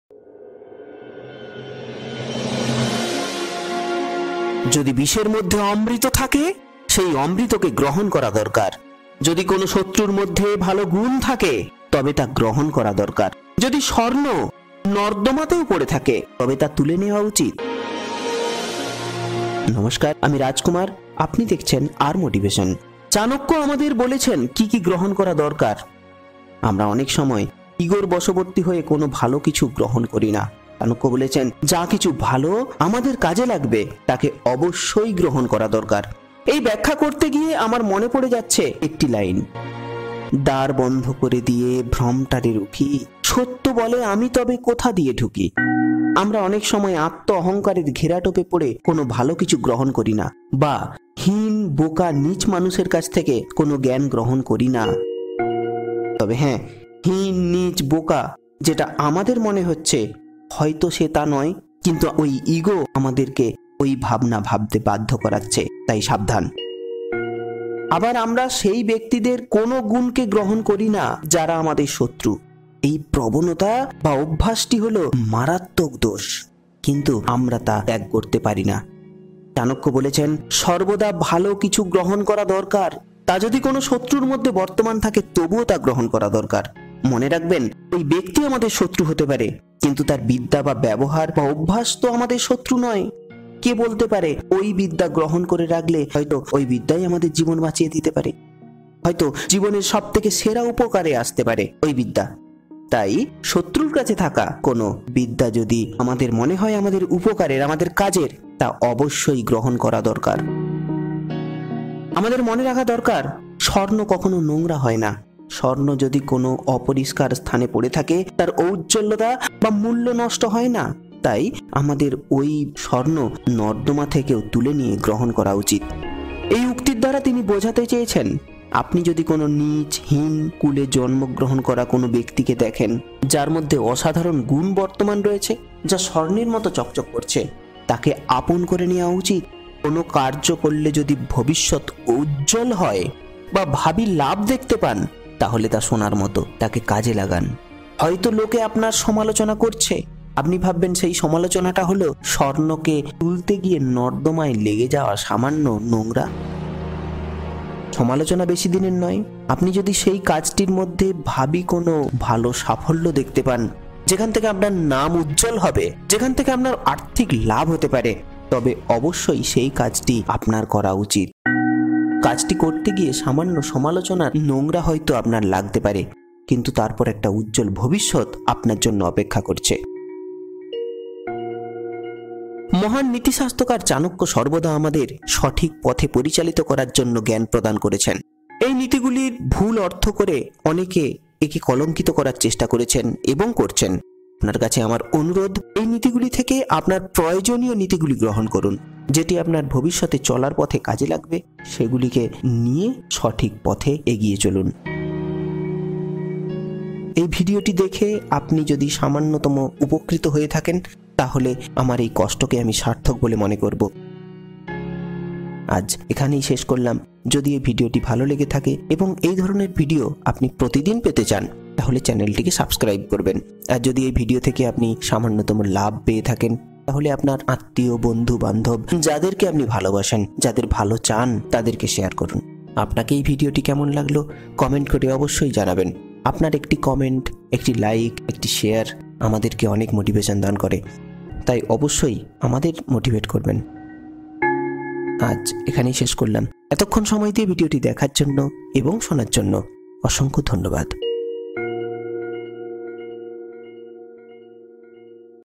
চাণক্যের উপদেশ । Chanakya Neeti in Bengali | Summary by R Motivation
আজ চানক্য নীতির উপর একটি ব্যখ্যামূলক ভিডিও আপলোড করলাম. আপনি কি কি গ্রহণ করবেন, এই বিষয়ে সম্পূর্ণ জ্ঞান আপনি এই ভিডিও থেকে পেয়ে যাবেন.
Chanakya Neeti in Bengali. Chanakya was an Indian teacher and great philosopher.we known him as Kautilya and Vishnugupta. Chanakya was the advisor of Chandragupta Maurya. I hope this video will help you for your knowledge.

Thanks
Rajkumar Chakraborty